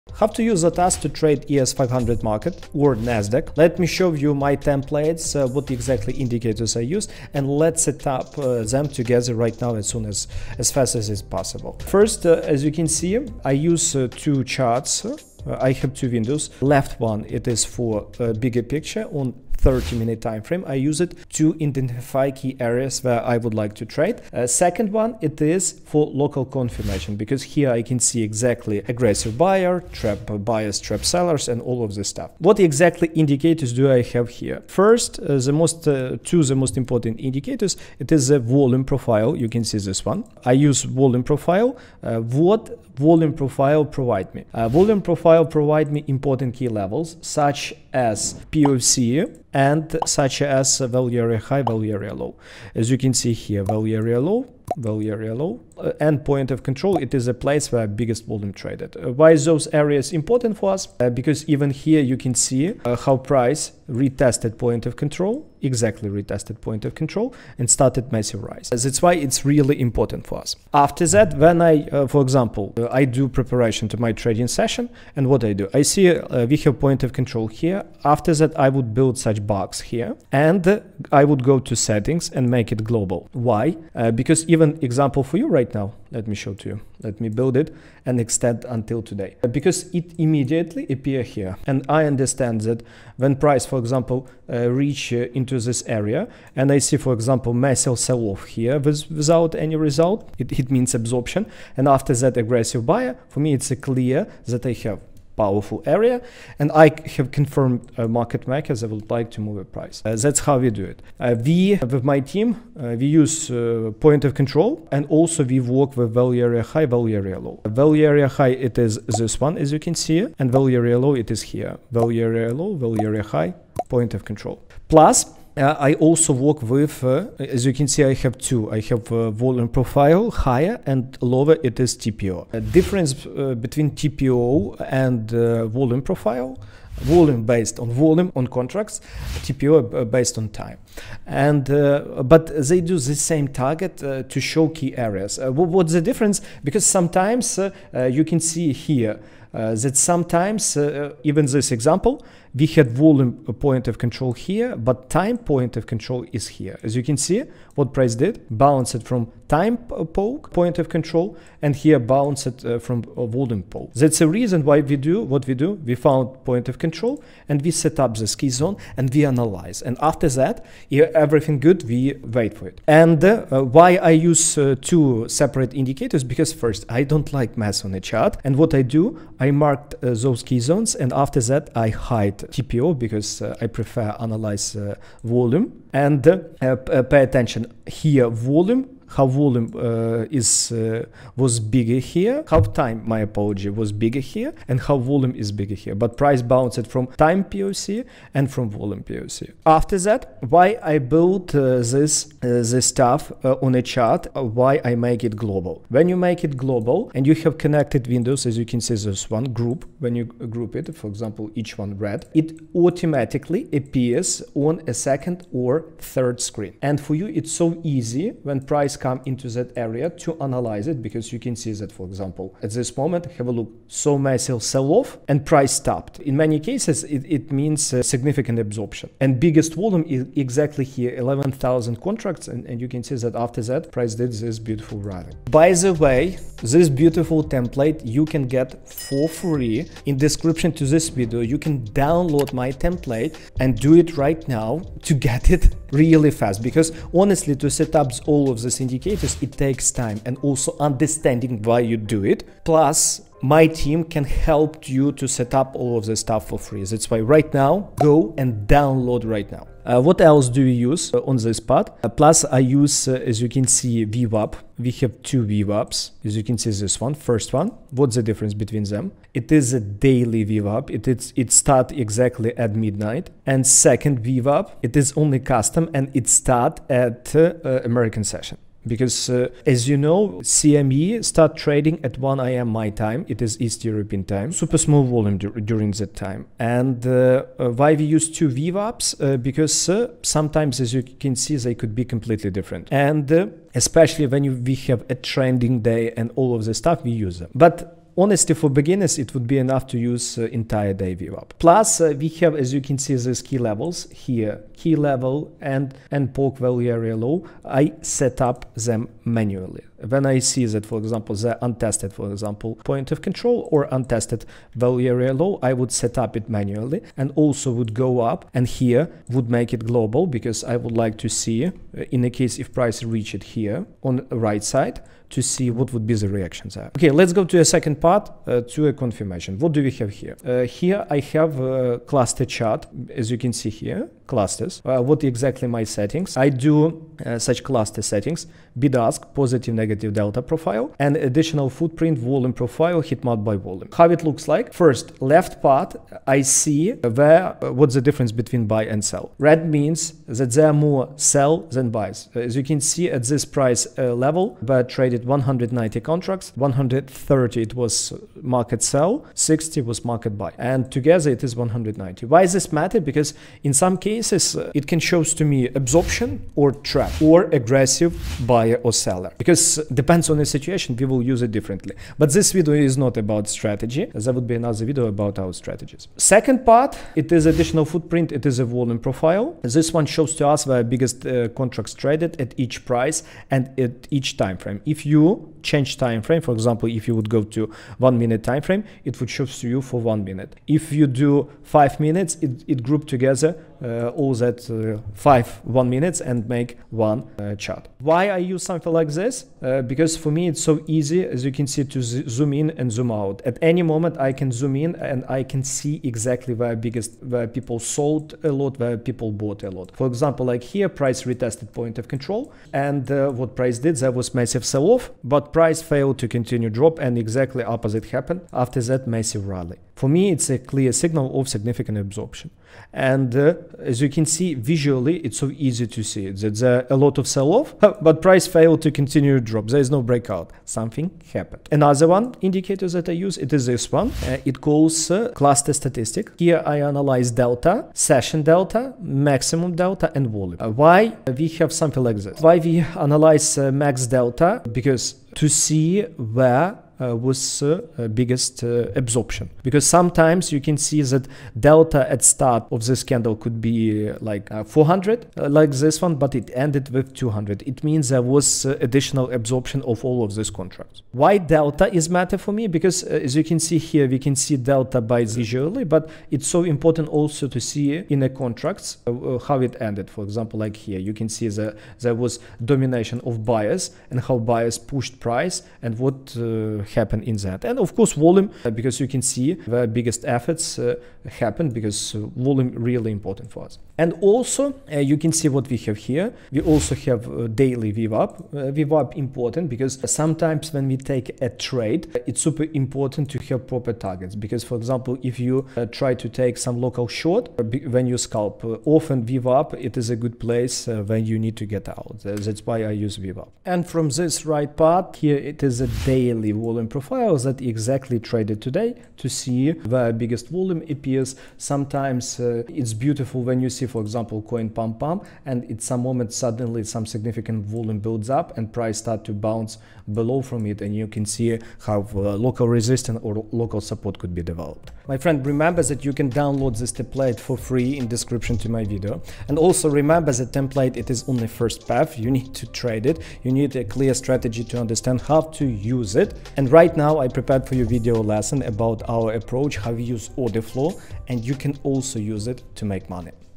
ATAS for ES500 and Trading | Trade CME with MY Templates
Timesmaps:
00:00 - Introduction
00:15 - why do I use 2 charts open for trading CME on ATAS?
00:40 - what does the volume profile indicator provide me?
01:00 - why the “point of control” zone is important for us
02:15 - what is the difference between TPO indicators and volume profiles?
02:55 - why and how do I use these two separate indicators on the ATAS platform?
03:20 - why do I turn on the global mode in the marked area on the chart on the ATAS platform
04:25 - two types of VWAP indicators that I use  to trade CME
04:50 - why I use two VWAP indicators in trading
05:05 - setting up key levels on the ATAS platform  to trade CME
05:35 - my setup for clusters on the ATAS platform  to trade CME
06:20 - what does the Volume Profile look like for different time periods?
07:10 - my settings for the Сluster Statistics indicator on the ATAS platform
07:40 - why is the Delta indicator important for me?
08:05 - daily VWAP indicator
08:25 - indicator Daily volume profile on the ATAS trading platform